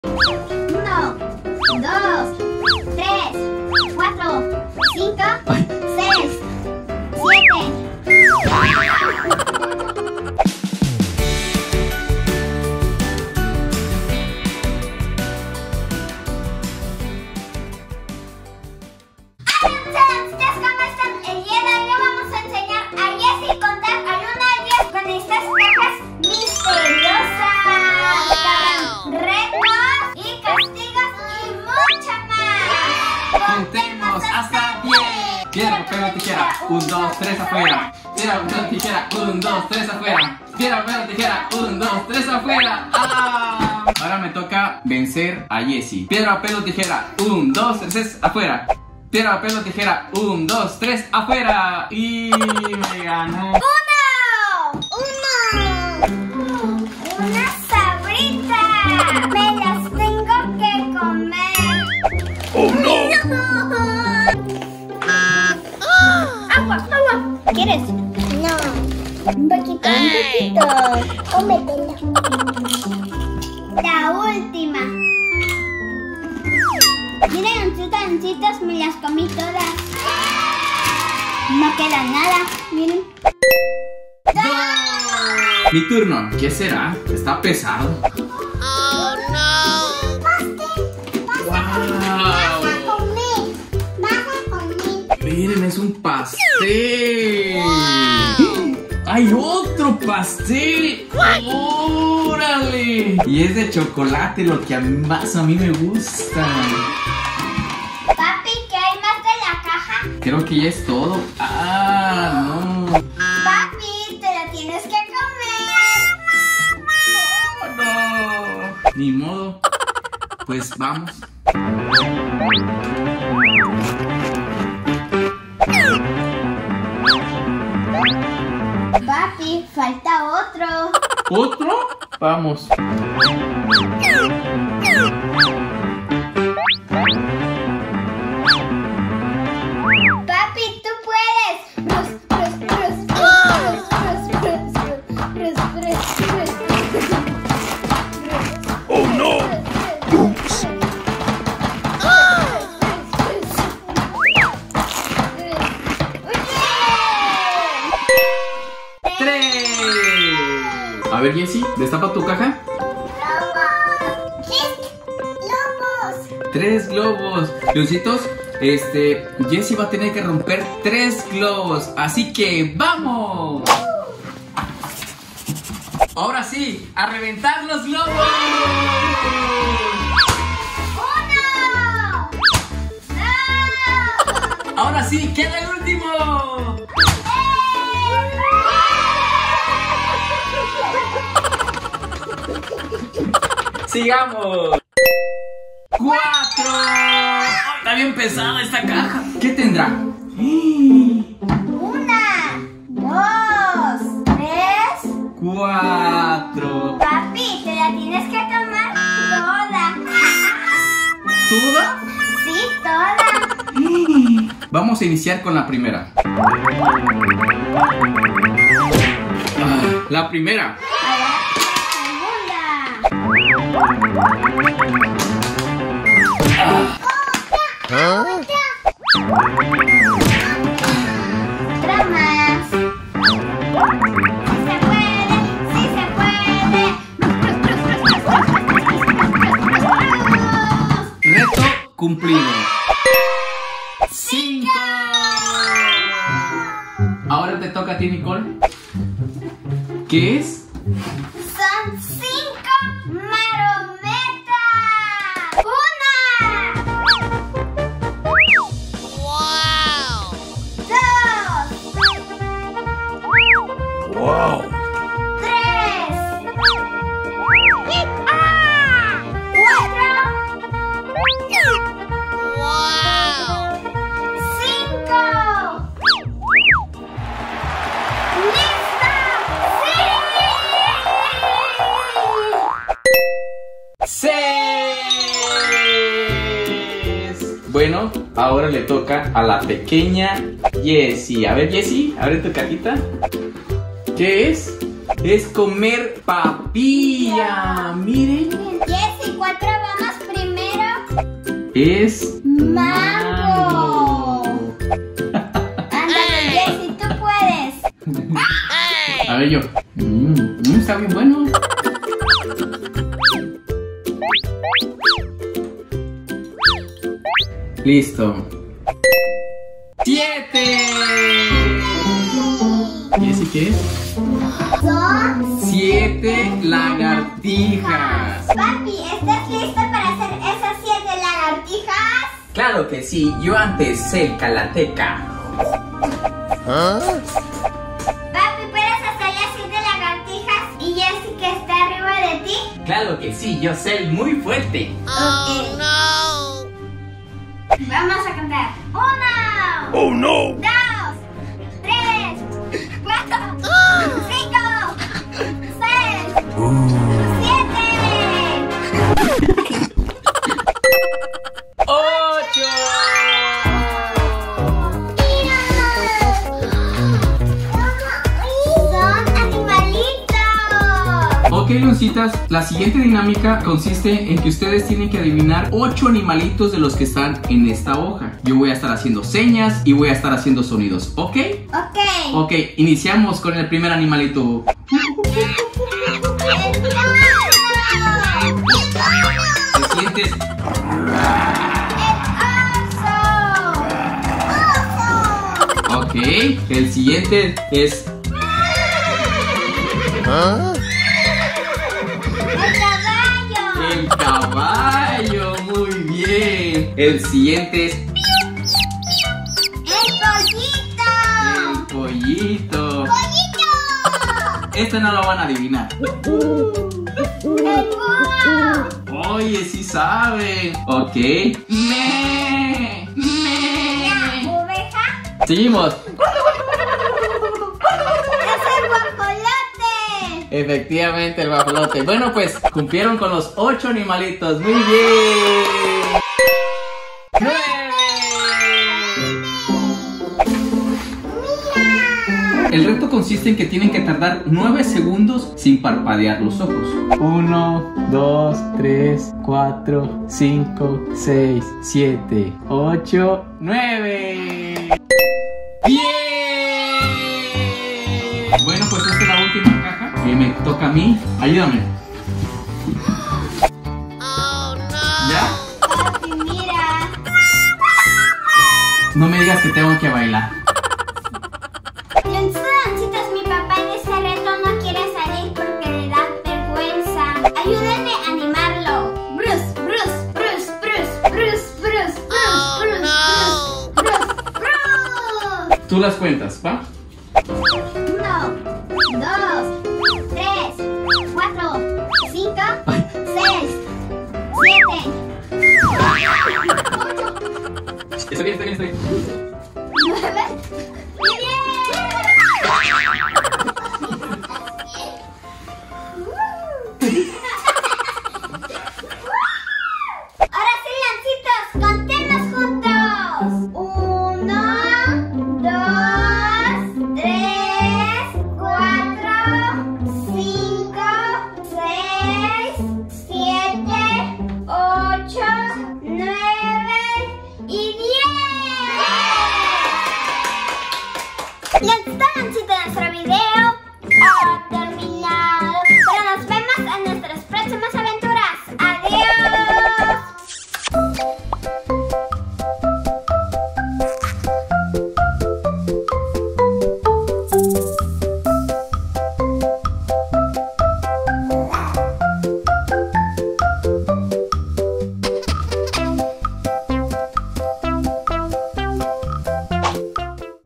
LAS LEONCITAS CONVIRTIERON A SU PAPA EN BAILARINA | Las Leoncitas Kids
Jugamos con nuestro Papa juegos y retos, y el perdió y se tuvo que vestir de bailarina jajajajaja se vio muy gracioso, mira el video 😂